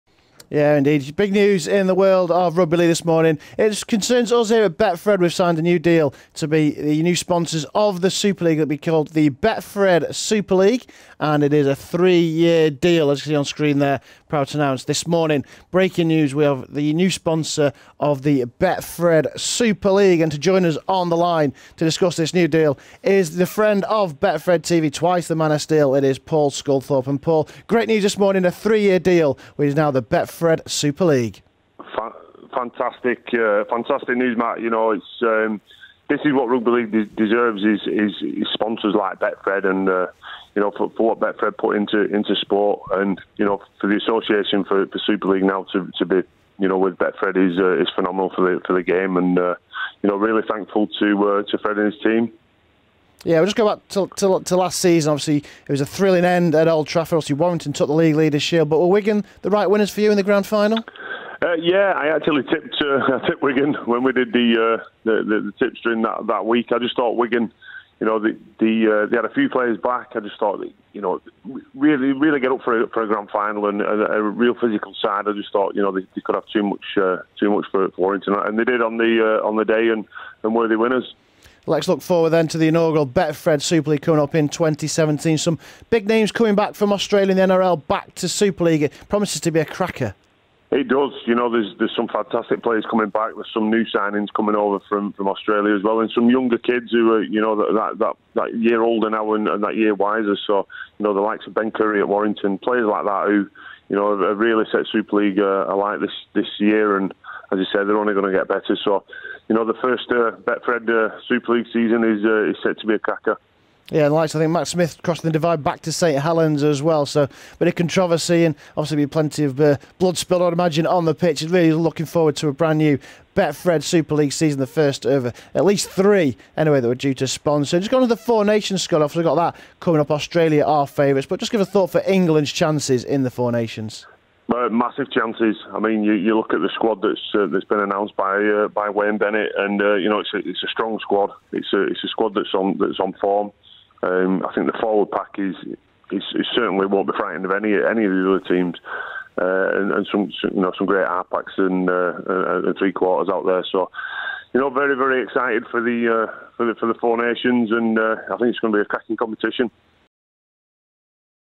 Paul Sculthorpe reacts to Rugby League announcement
Two-time Man Of Steel Paul Sculthorpe reacts to our announcement of a 3 year sponsorship deal with the Super League | You'll love a bit of Betfred